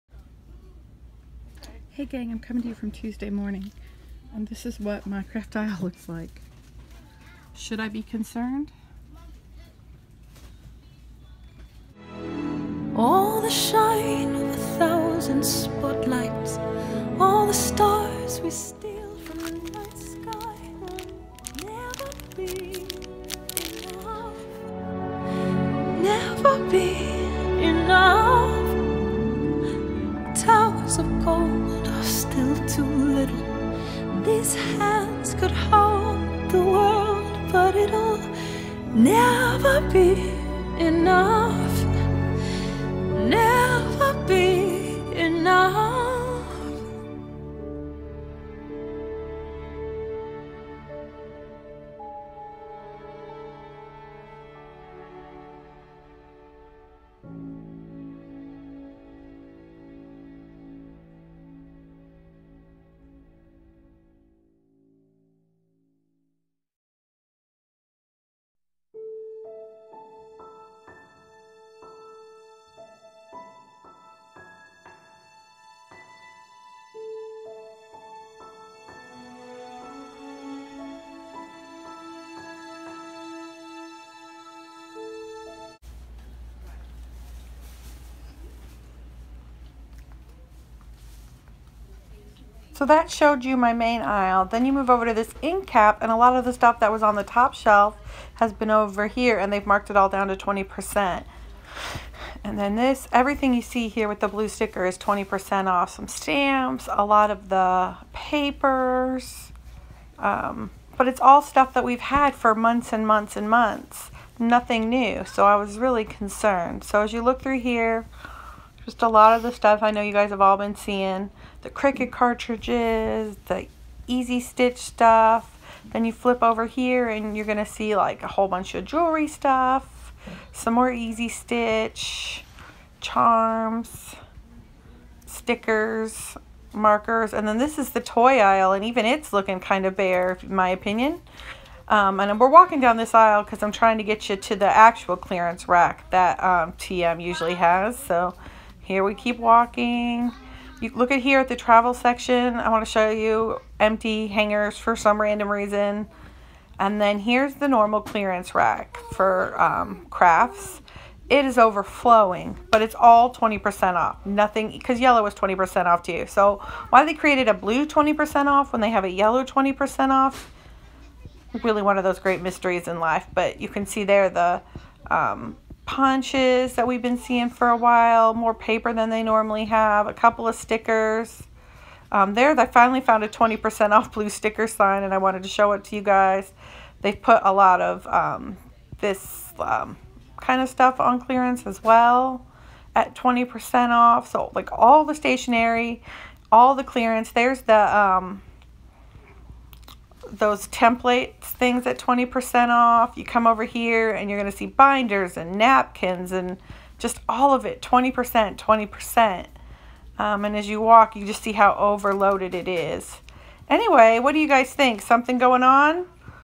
What is going on Tuesday Morning
So I went to Tuesday Morning Saturday and this is what I found!